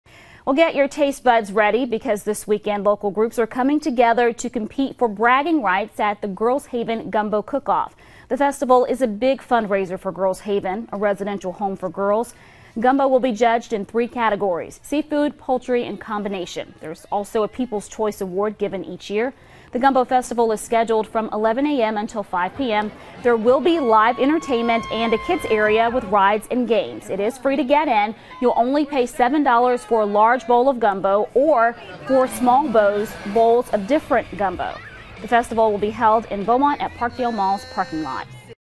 Girls' Haven prepares for Gumbo Festival
Get your taste buds ready because this weekend, local groups are coming together to compete for bragging rights at the Girls' Haven gumbo cook-off.            The festival is a big fundraiser for Girls' Haven, a residental home for girls. Gumbo will be judged in three categories:  Seafood, poultry and combination.  There is also a People's Choice Award given each year.  The gumbo festival is scheduled from 11 a.m. to 5 p.m. There will be live entertainment and a kid's area with rides and games.It's free to get in.You'll only pay $7 for a large bowl of gumbo or four small bowls of different kinds of gumbo.The festival will be held in Beaumont at Parkdale Mall's parking lot.